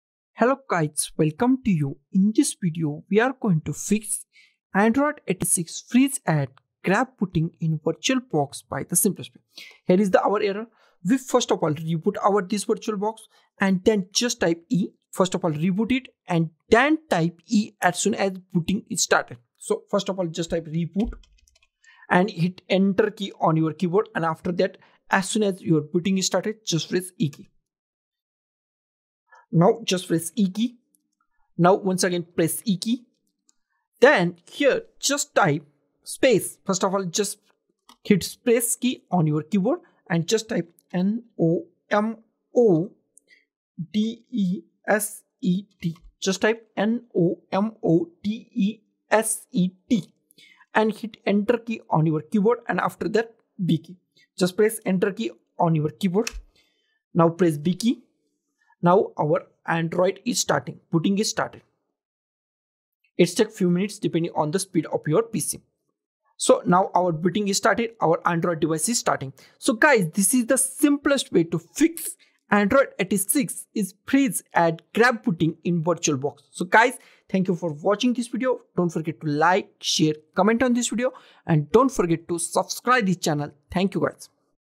How to Fix Androidx86 Freezes at GRUB Booting in VirtualBox
In this Video we are going to see How to Fix Androidx86 Freezes at GRUB Booting in VirtualBox by the Simplest Way.